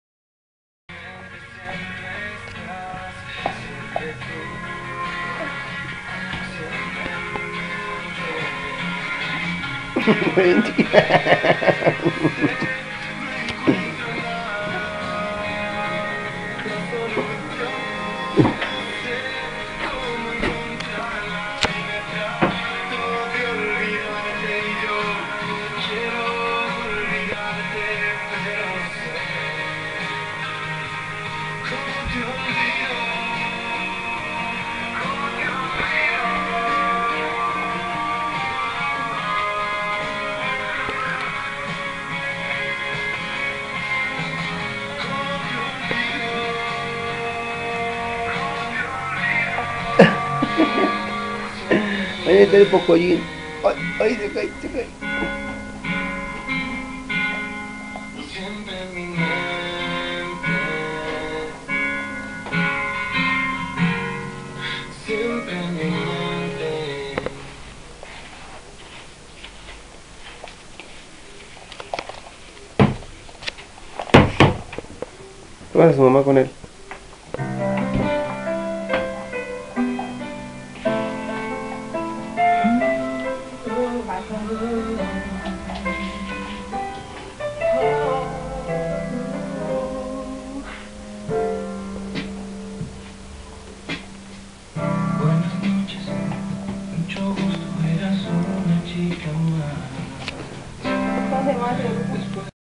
pelon y su hijo mi perrito
apenas se enseñaba a sentar